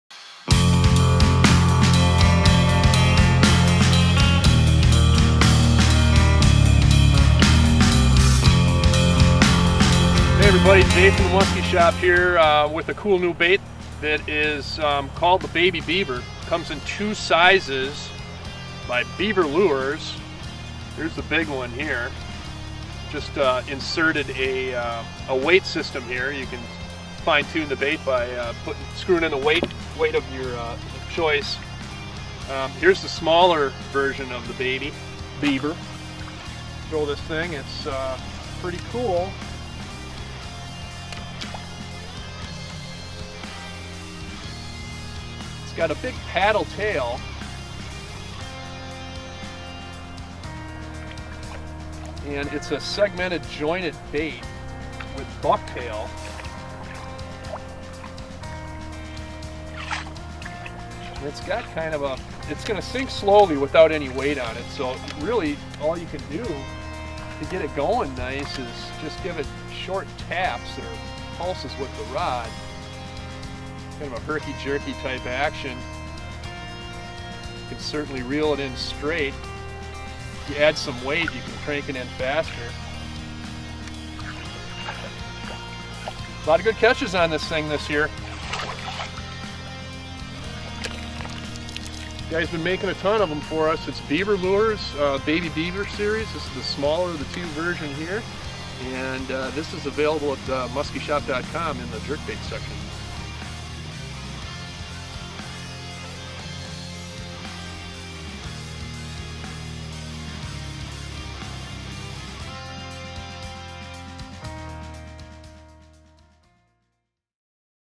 Baby Beaver Bait with Jay from the Musky Shop
The Baby Beaver is constructed of plastic with .051 wire molded into each body piece. Each body piece is hand tied with deer hair and connected together with split rings. The rubber tail is molded onto the hook and is attached to the bait with a split ring making it replaceable.The Beaver is a unique blend of rubber and hair providing a swimming action during retrieve that is an almost irresistible target for a hungry Musky.